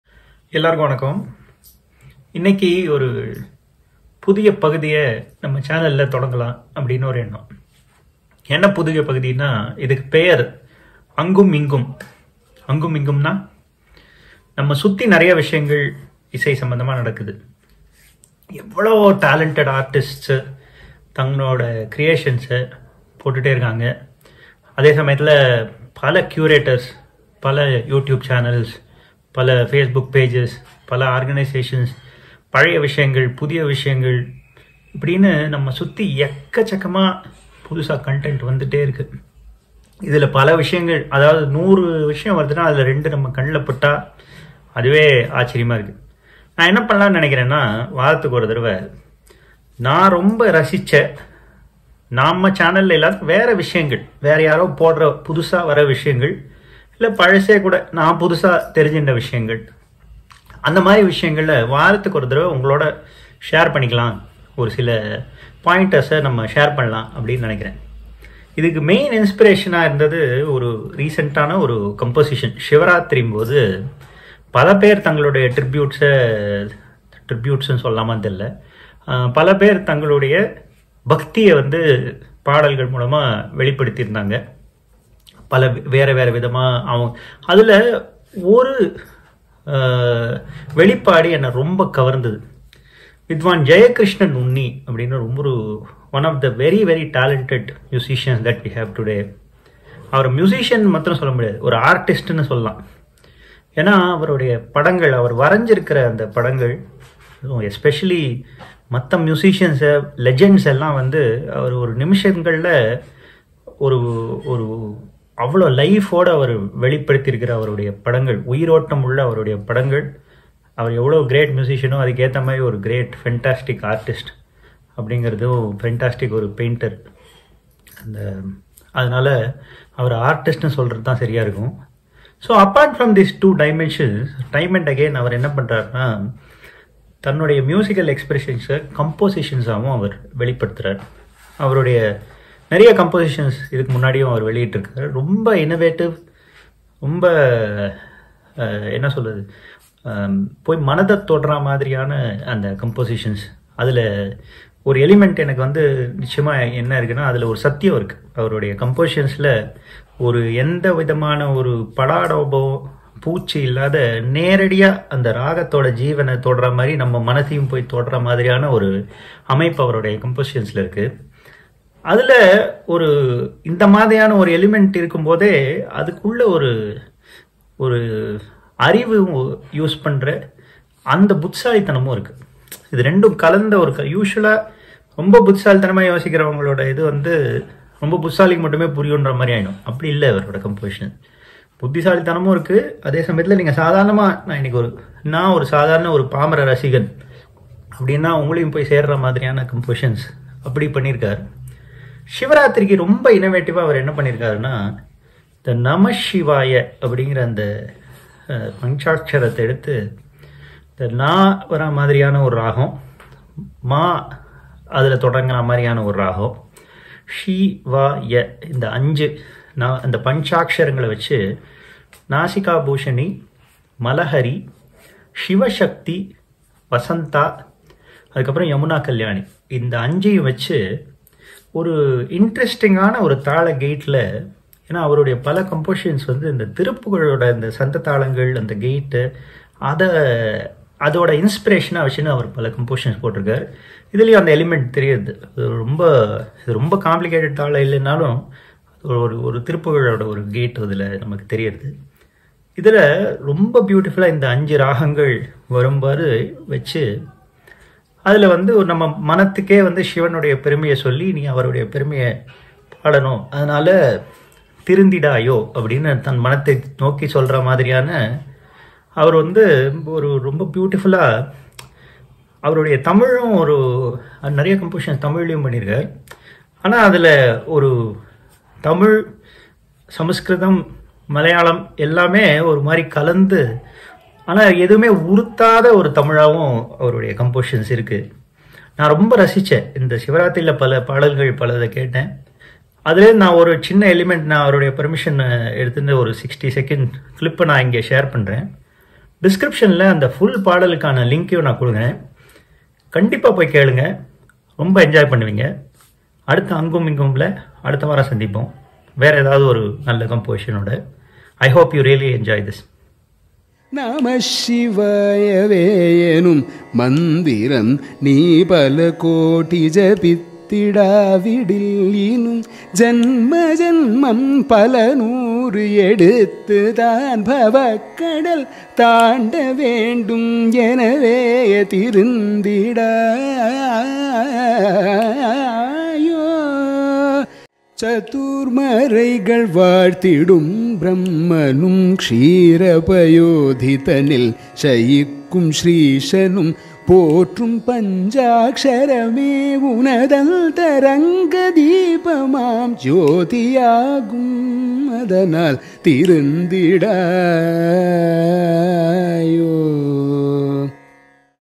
Angum Ingum - 1 - N Jayakrishnan
In this new weekly series, I bring to you the content on the internet I enjoyed the most. The intention is to curate good content that is been around.

In this first video, I'm sharing a wonderful composition of the talented artist N Jayakrishnan.